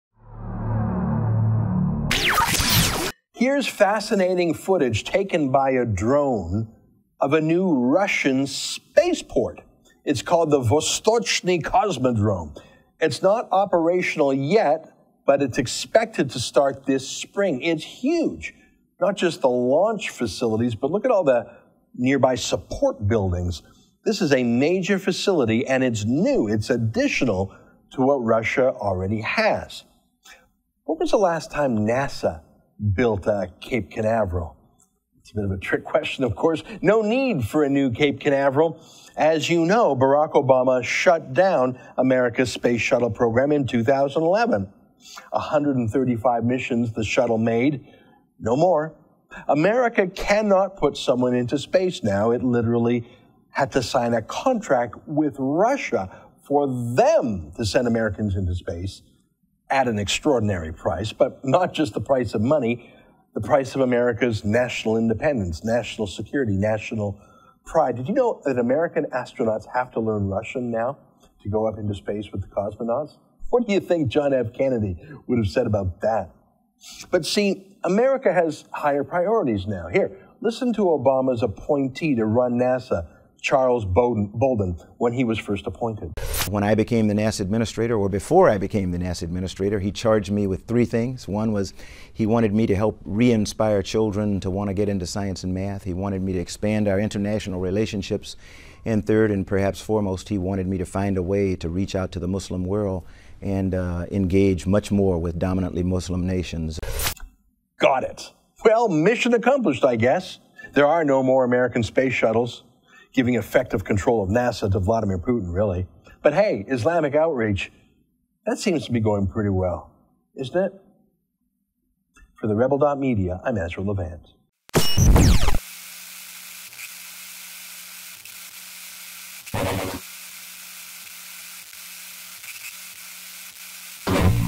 New Russian spaceport a reminder of Obama's abandonment of NASA
Ezra Levant of TheRebel.media reports: The new Russian spaceport, the Vostochny Cosmodrome, will be operational this spring. When was the last time NASA built a Cape Canaveral? That's a trick question, of course: Under Obama's watch, NASA was told to focus on "Muslim outreach, remember? MORE